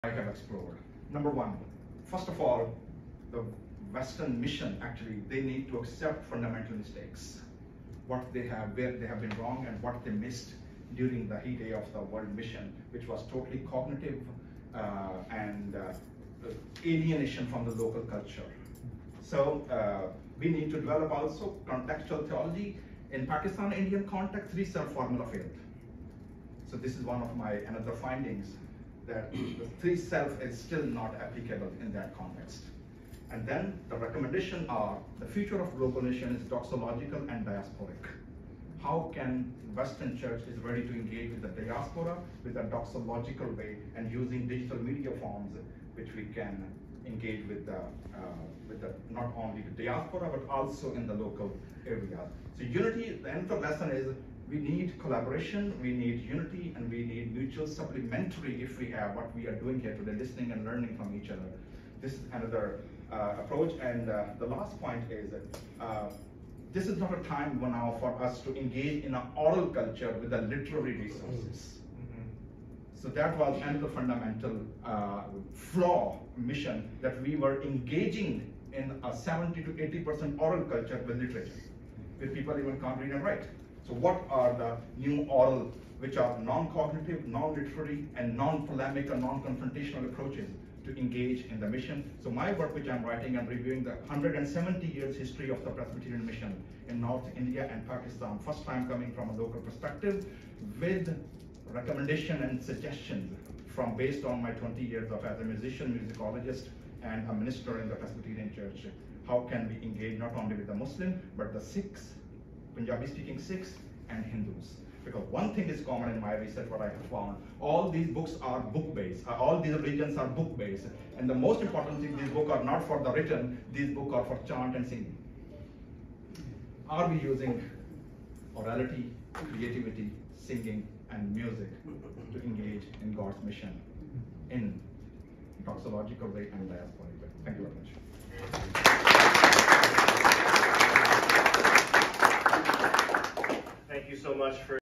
Presbyterians & Fundamental Flaws of the Foreign Missions
This lecture is presented with collaboration of Overseas Ministries Study Centre to the faculty of Princeton Theological Seminary. Rev.Dr. Eric Sarwar sharing his research on the History of Presbyterian Mission in North India and Pakistan and the importance of doxological and diasporic approaches in the 21 st century.